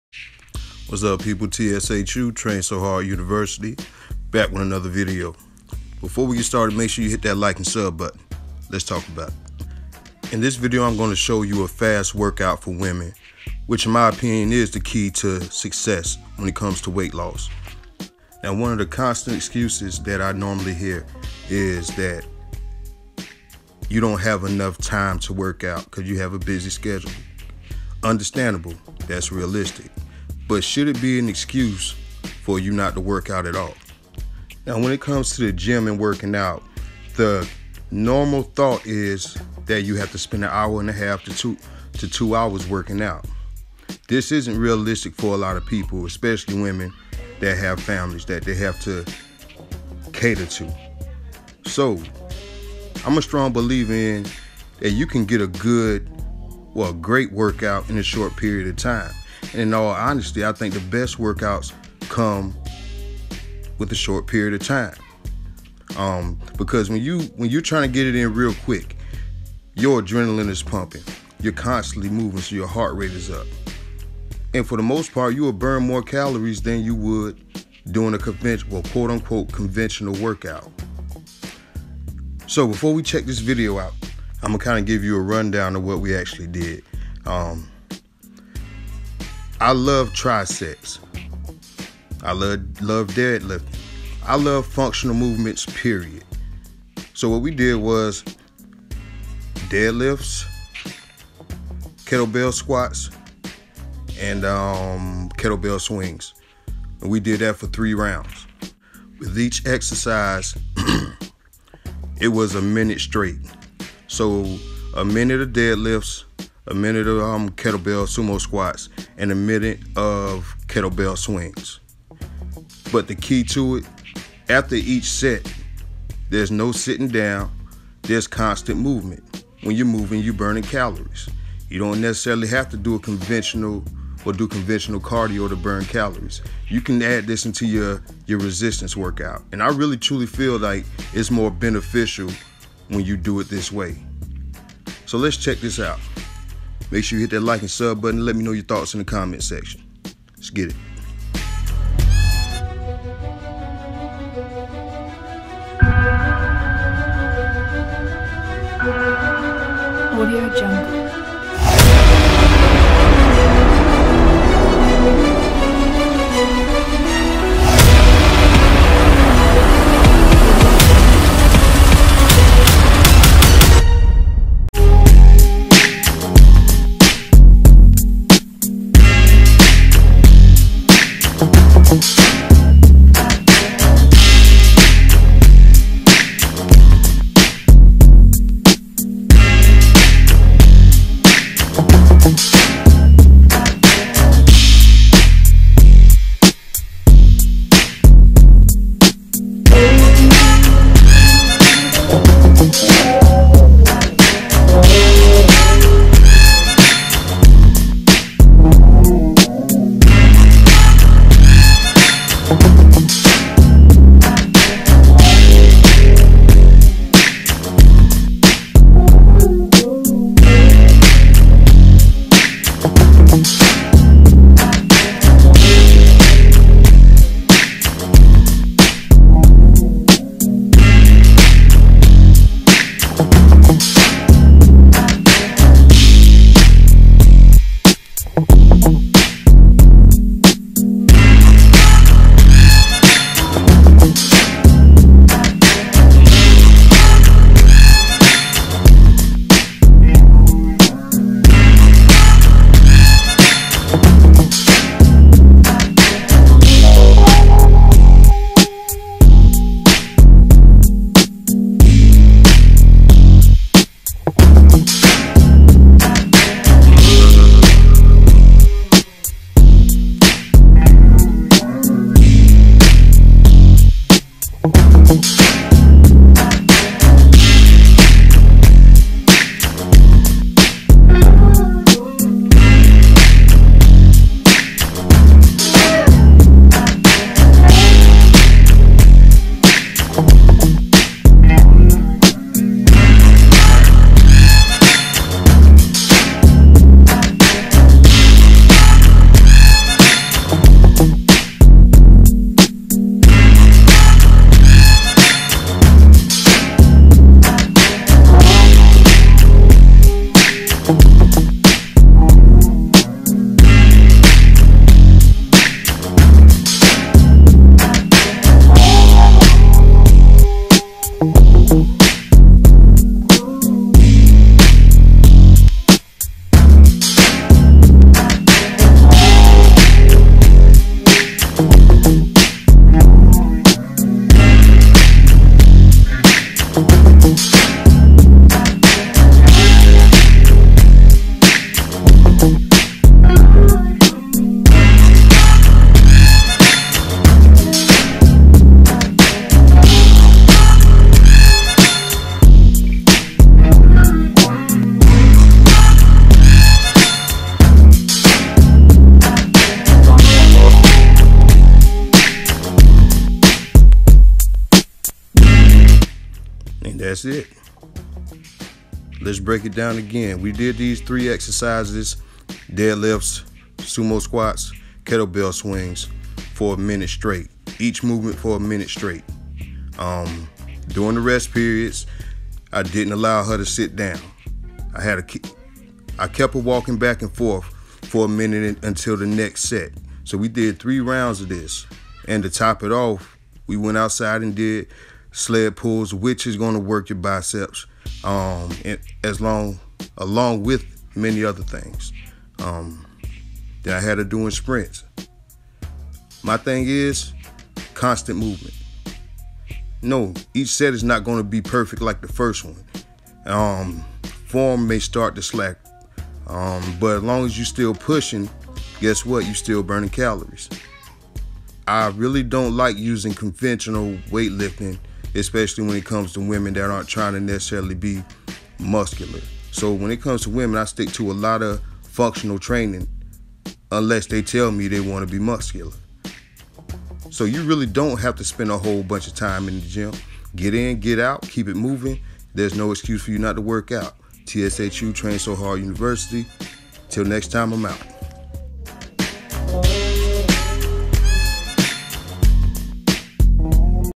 GYM WORKOUT FOR WOMEN. THE KEY TO FITNESS RESULTS
FAST WORKOUT FOR WOMEN THE KEYS TO YOUR WEIGHT LOSS SUCCESS.  A majority of us have busy lives.  When you’re busy, we tend to neglect many things.  The one thing we shouldn’t neglect is our health.  So for the busy ladies this a fast and effective workout that will get you results.